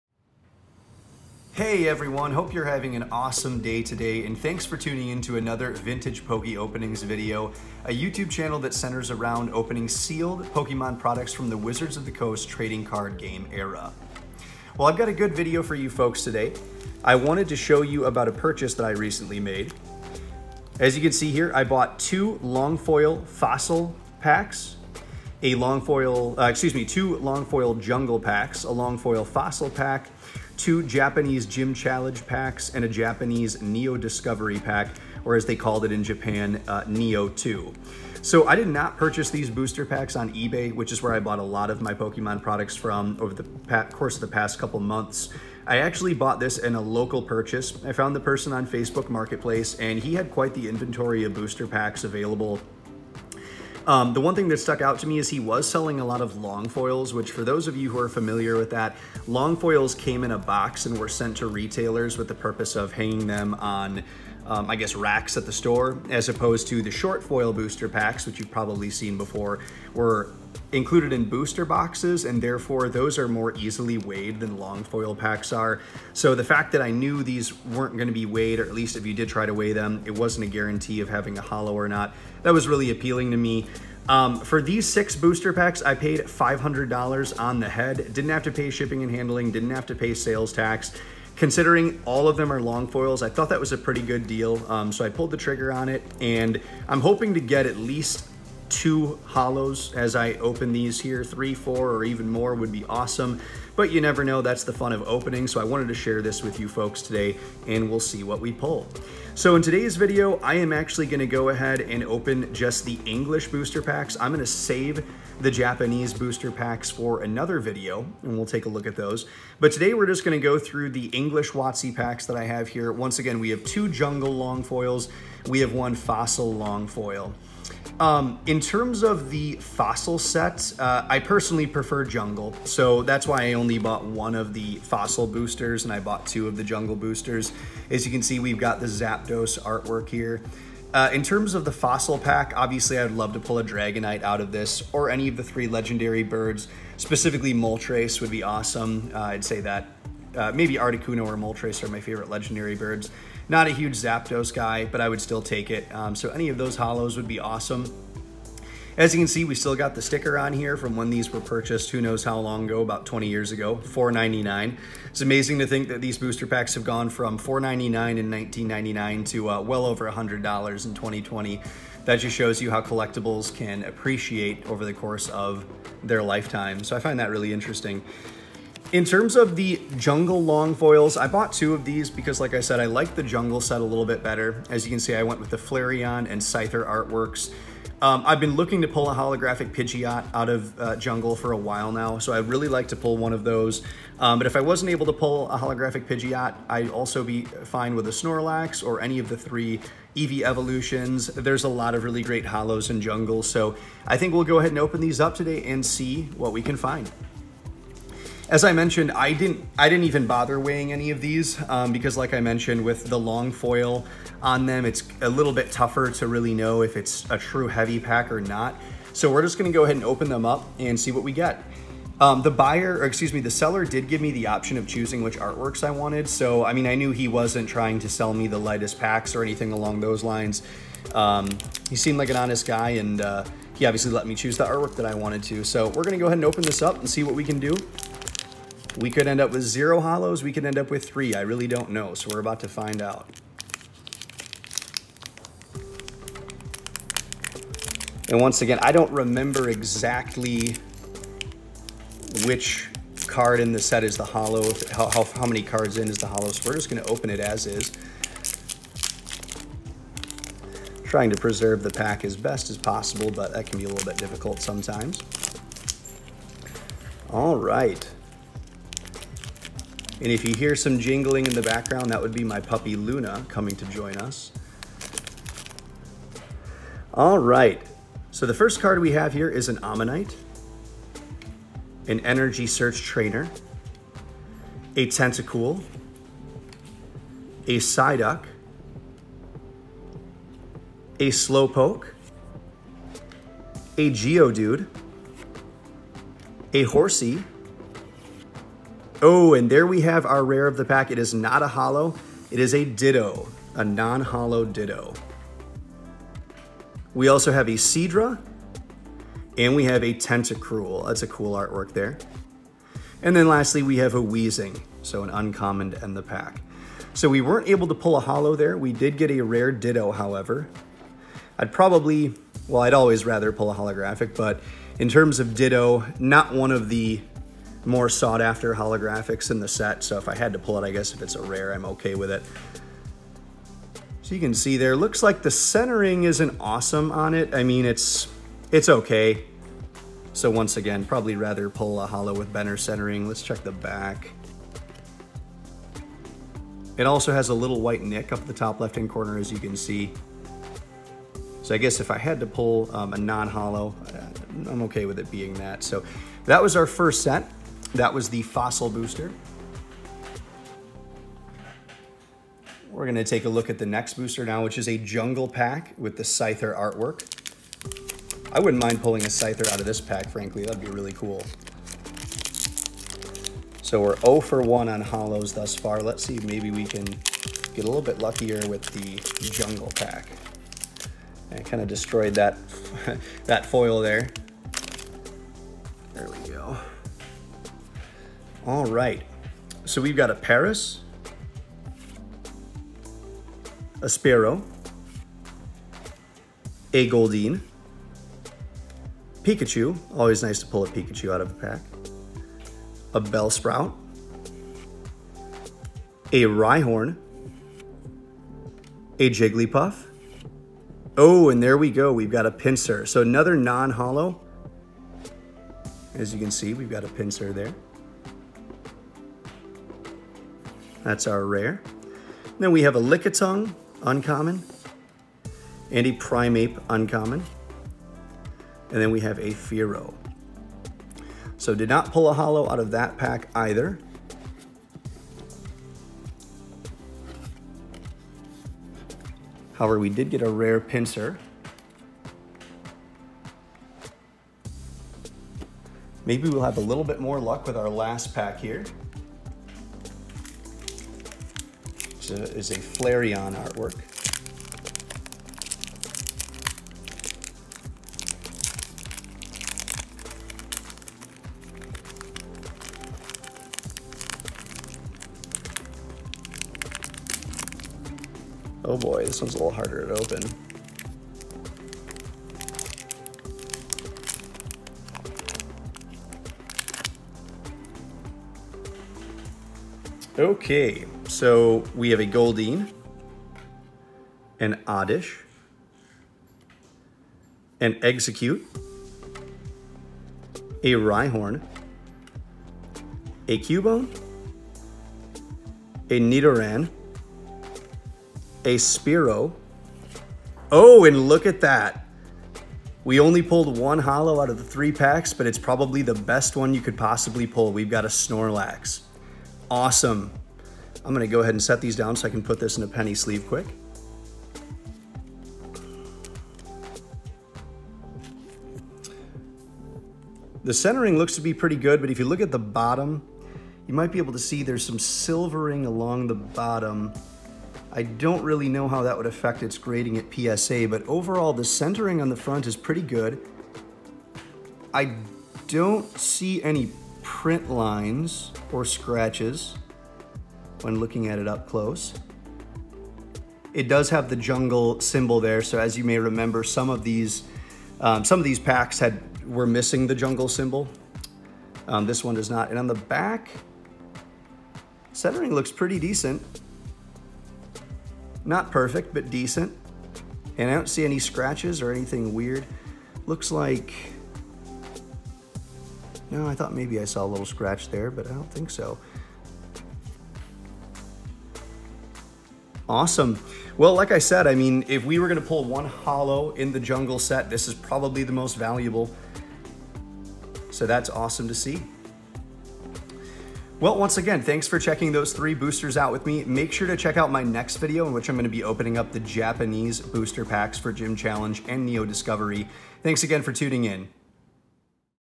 3x Vintage WOTC Pokémon Booster Packs! | Jungle and Fossil
I recently purchased some Jungle and Fossil booster packs from a local seller. Watch the video to see what I pulled!

How to Submit Pokémon Cards to PSA? | PSA Submission & PSA Returns